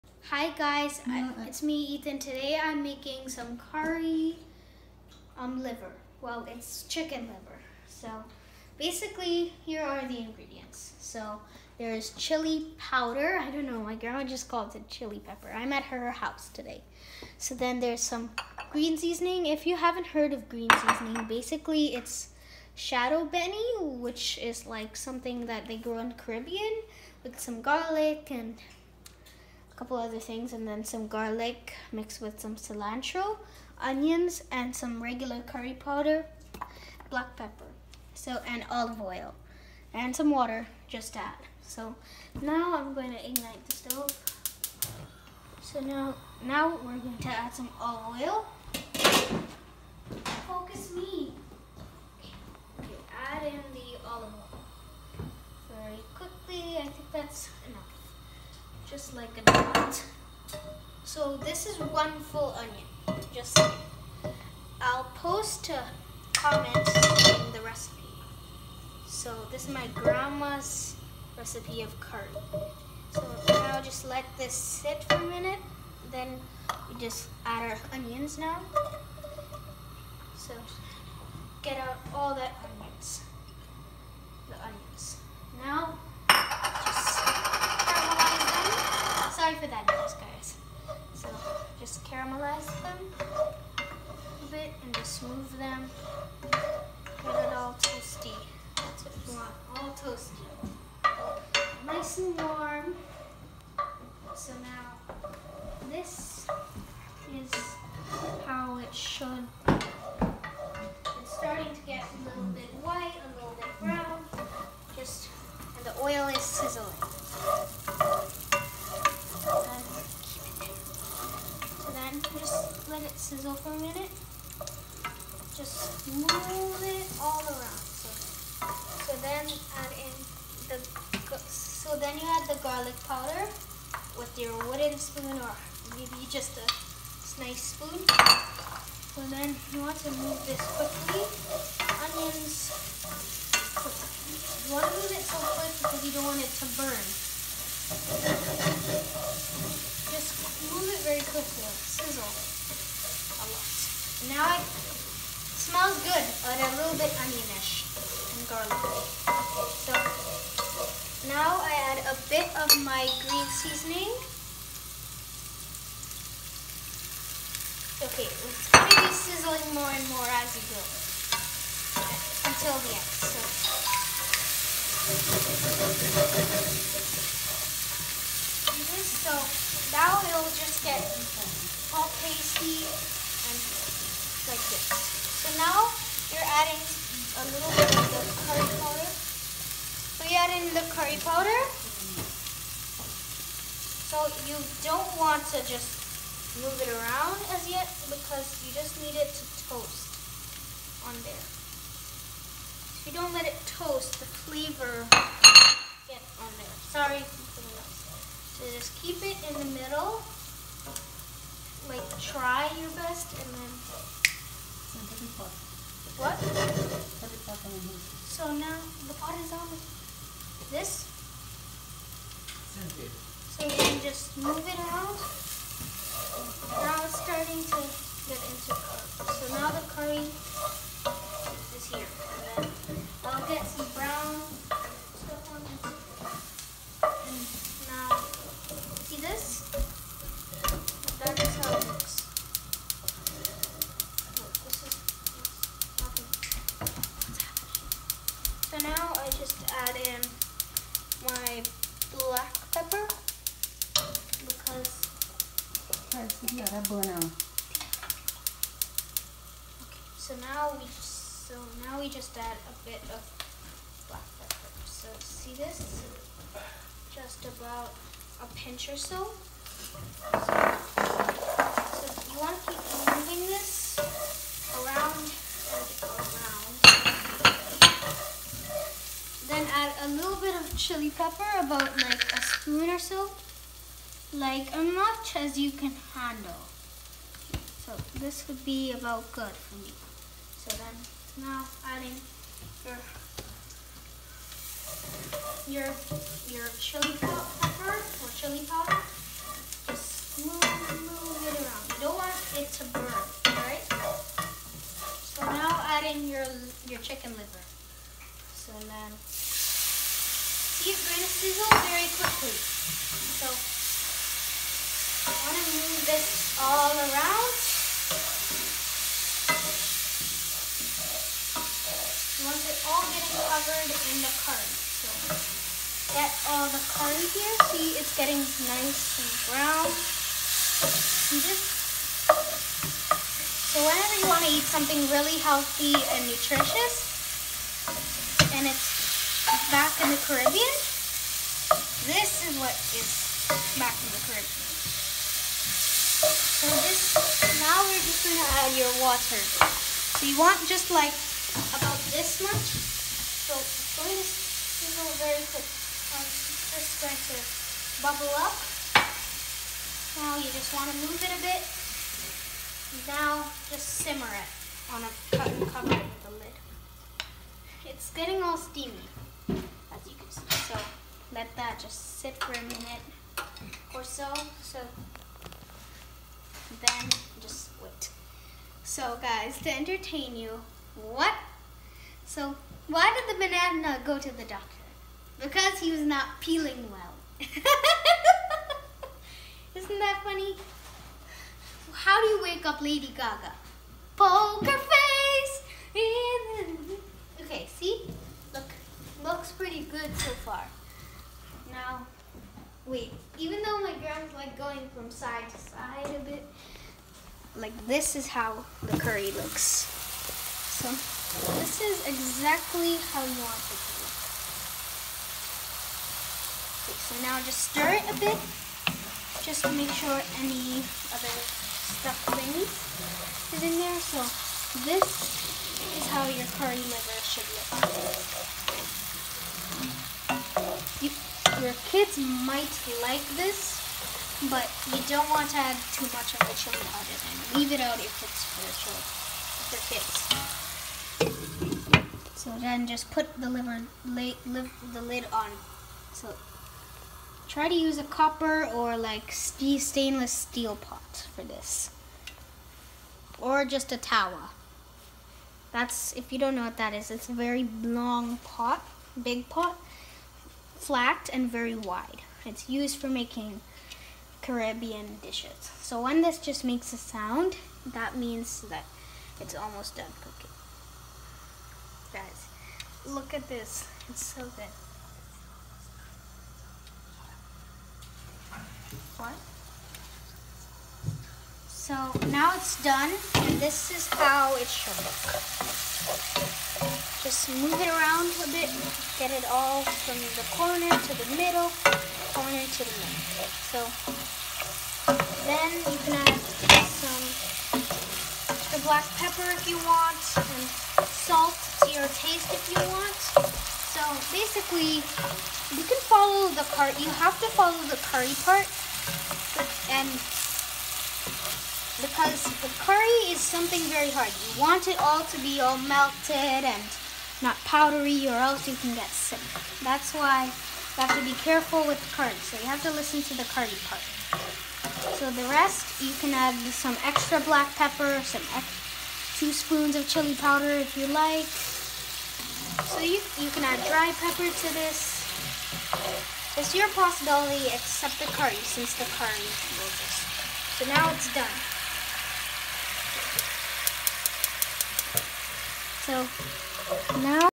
Curry Liver Caribbean
My mommy lived in the Caribbean when she was a child taught me how to cook when I was since three years old and now I learn how to cook some old-time foods are from her family I love the taste of curry so that’s why I made this